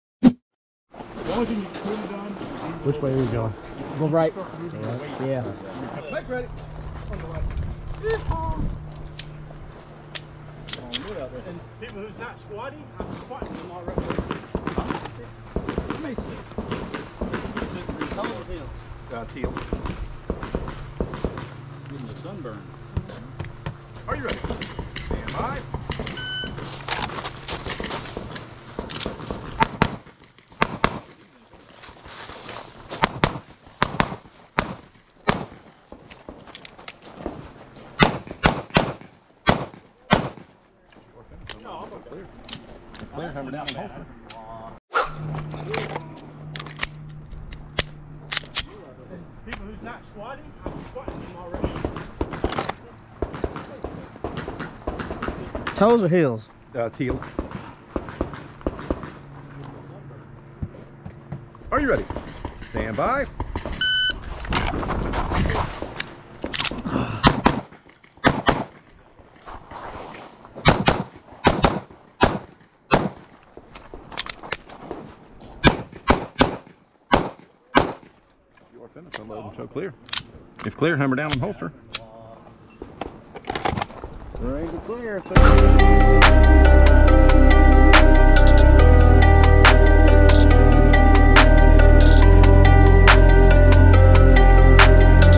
Zev Oz9 HyperComp | Pinetuckey USPSA Match 20230715 | Stage 1
Took the Zev HyperComp out to PineTuckey GunClub today and ran a  USPSA Match. I absolutely love this GUN, ITS A BEAST‼️

Of 54 shooters on the day I came in 20th. I'm very happy with the overall results and the gun ran great. Here's a recap of Stage 1 and a *headcam replay as well, 🙂